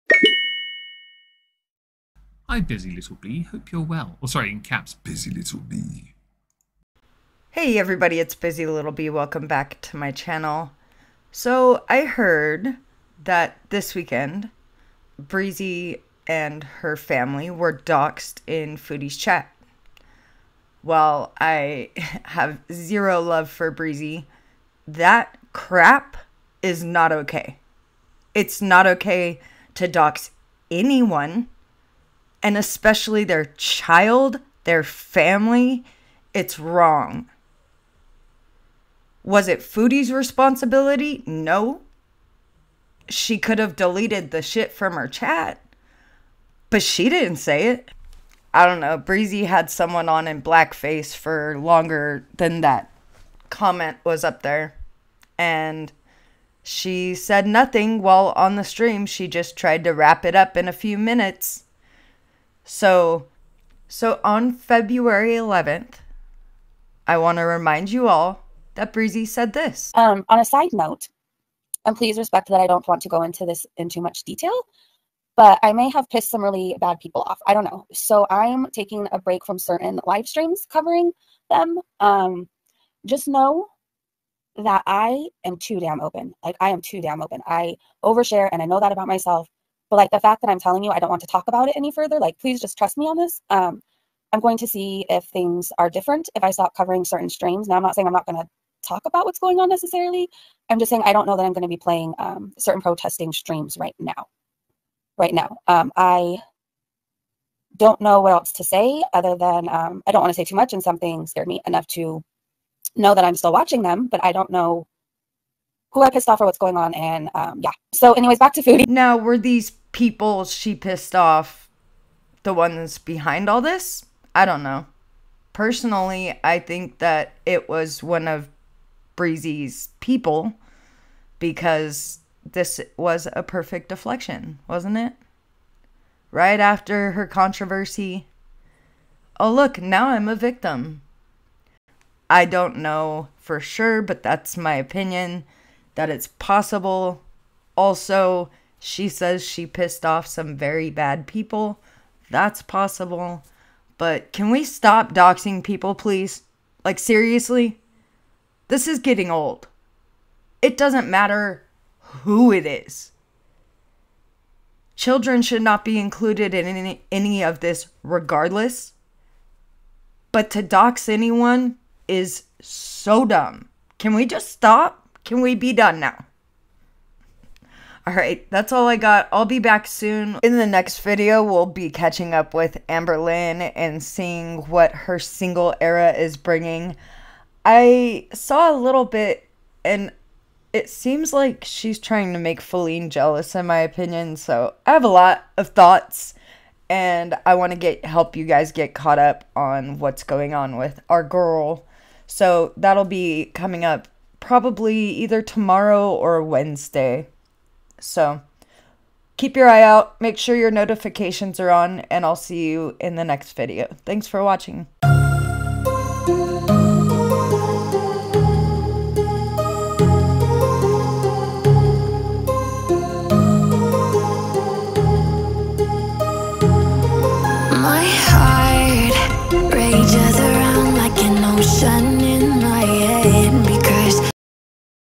BREEZIN DOXXED IN FOODIE BEAUTY'S CHAT- IS THIS WHY??
Who doxed Breezy and her family this past weekend in Foodie Beauty's chat?